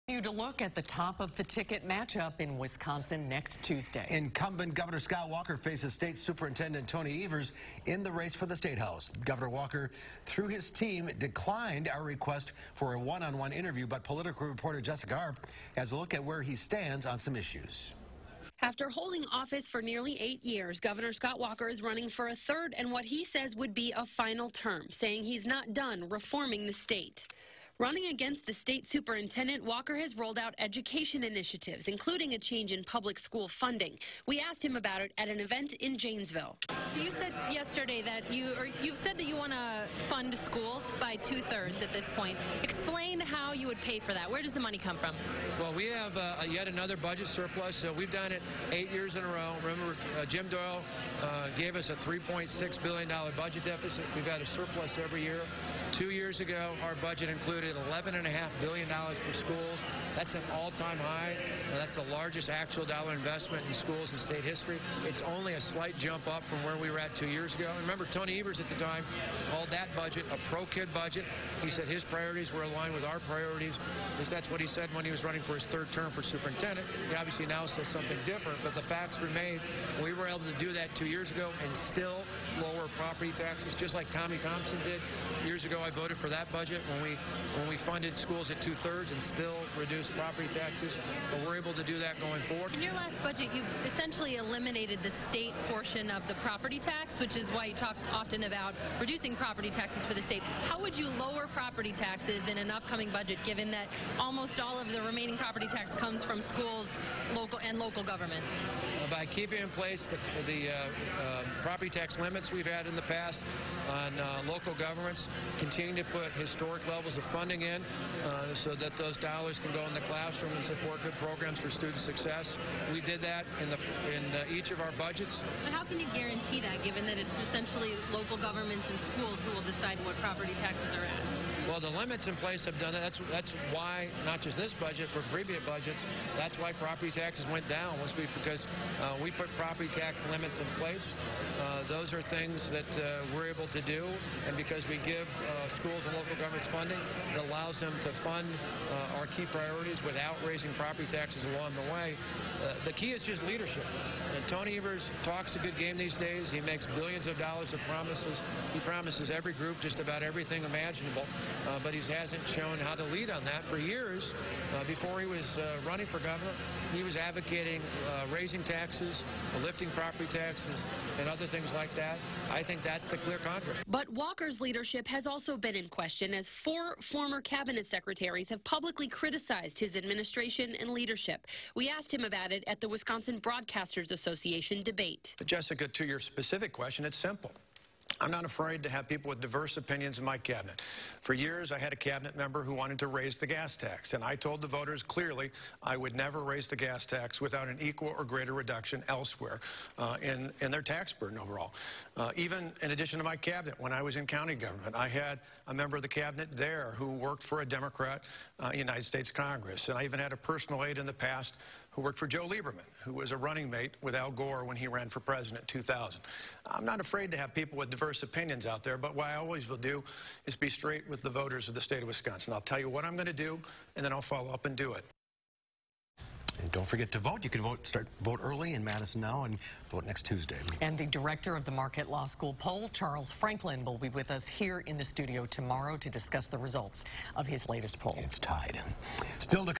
Campaign 2018: Scott Walker's take on issues facing governor candidates
Jessica Arp explains Gov. Scott Walker's take on the issues facing governor candidates.